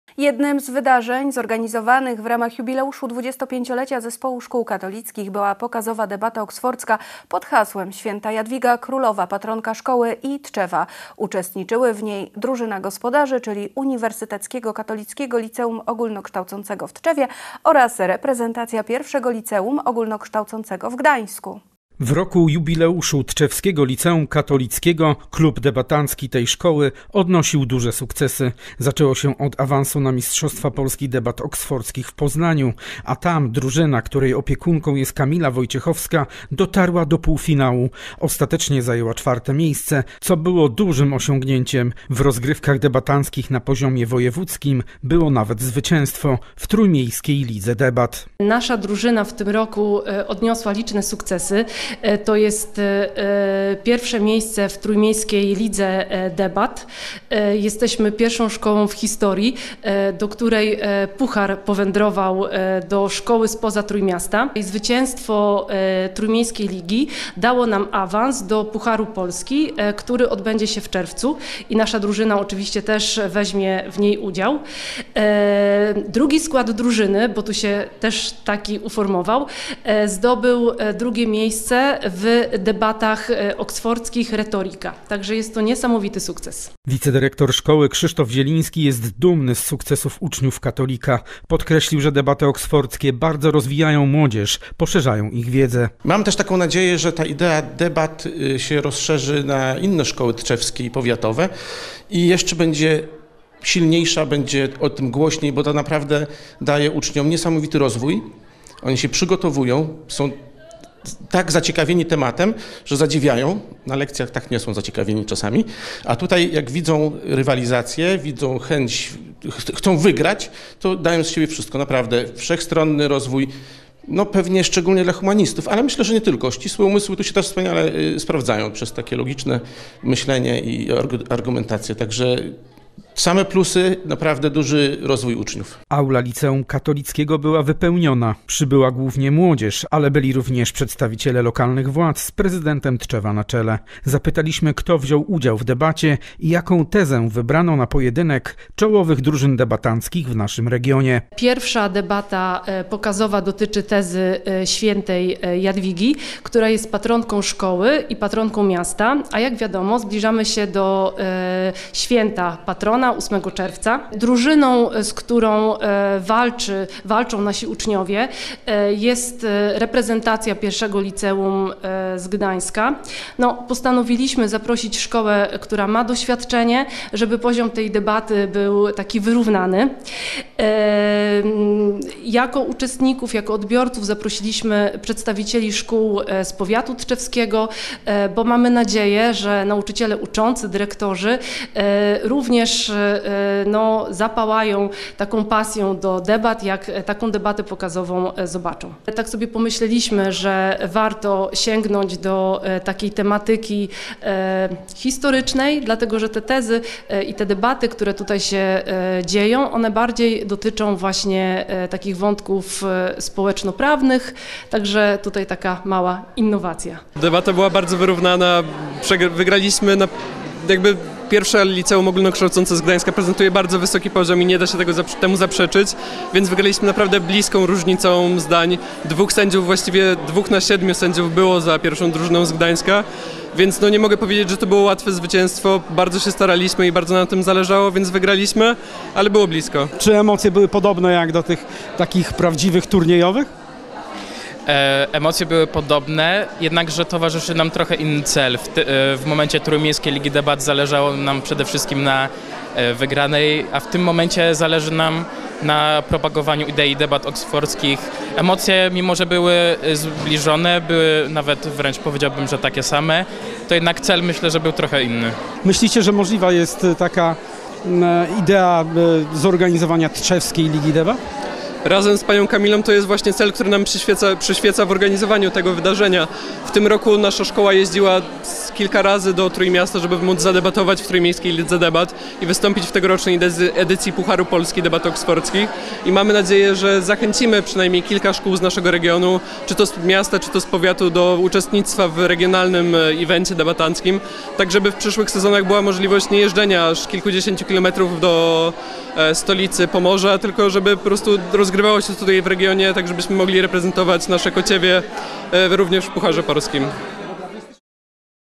Pokazowa debata oksfordzka w katolickim liceum - Tv Tetka Tczew HD
Jednym z wydarzeń zorganizowanych w ramach jubileuszu 25-lecia Zespołu Szkół Katolickich była pokazowa debata oksfordzka pod hasłem “Święta Jadwiga królowa - patronka szkoły i Tczewa”. Uczestniczyły w niej: drużyna gospodarzy, czyli Uniwersyteckiego Katolickiego Liceum Ogólnokształcącego w Tczewie oraz reprezentacja I Liceum Ogólnokształcącego w Gdańsku.

Jesteśmy częścią Telkab Sp. z o.o. - lokalnego dostawcy internetu i telewizji. Sprawdź naszą ofertę na